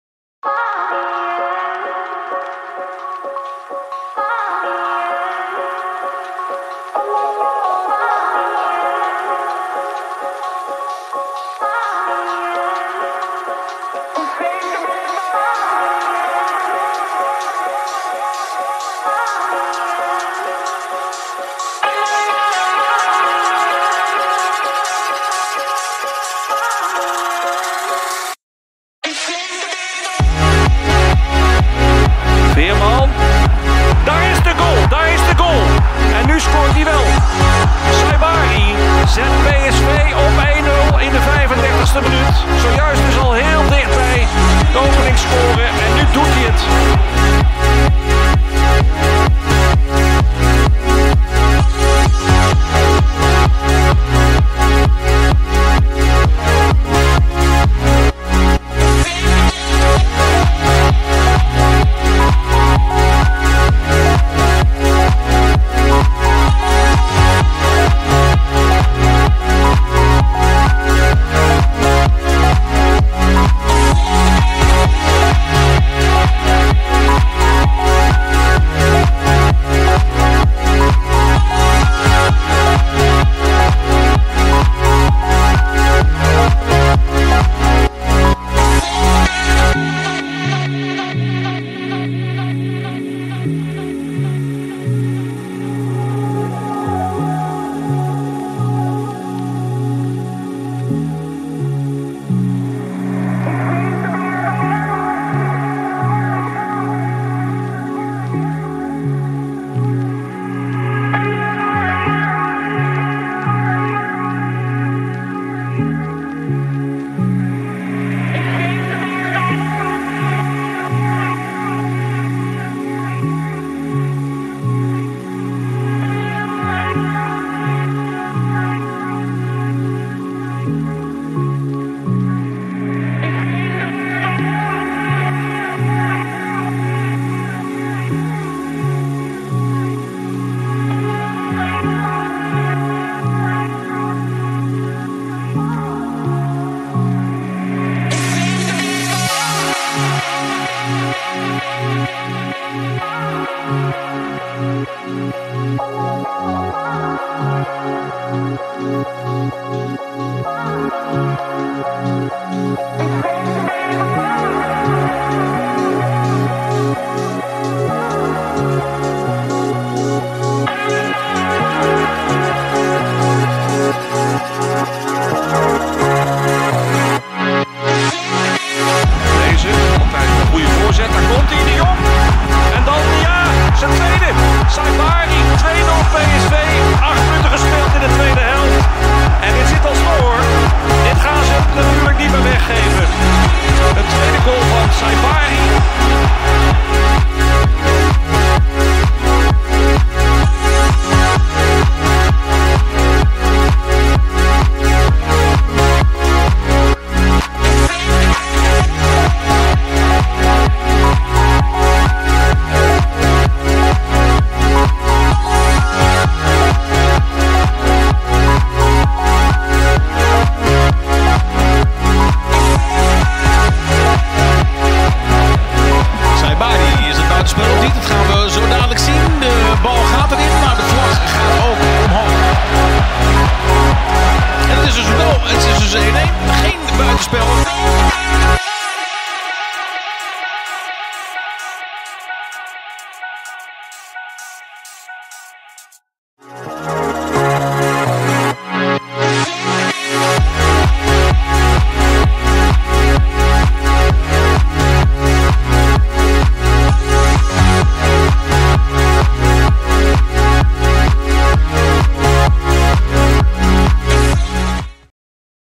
Ismael Saibari ►Unstoppable ● 2023/24 ● PSV Eindhoven ᴴᴰ
Ismael Saibari Ben El Basra (born 28 January 2001) is a professional footballer who plays as an attacking midfielder or a right winger for Dutch Eredivisie club PSV Eindhoven. Born in Spain, he is a youth international for Morocco.

Personal information
Full name: Ismael Saibari Ben El Basra
Date of birth: 28 January 2001 (age 22)
Place of birth: Terrassa, Barcelona, Spain
Height: 1.85 m (6 ft 1 in)
Position(s): Attacking midfielder, winger